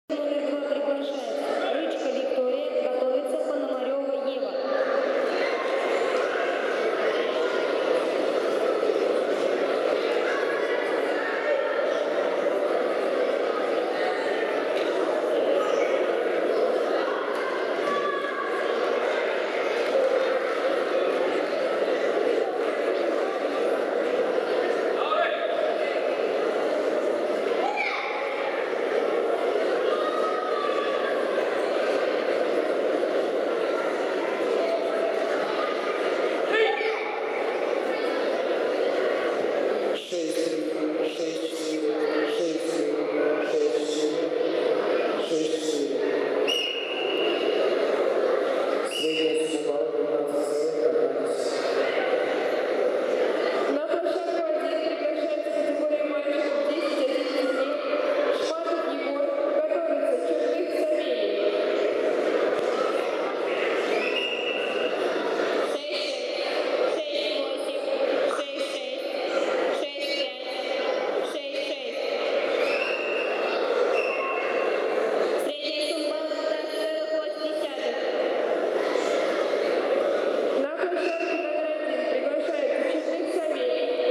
Рычка Виктория, ката Тайкоку Соно Ни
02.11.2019 Чемпионат ЮФО по каратэ кекусинкай, Волгоград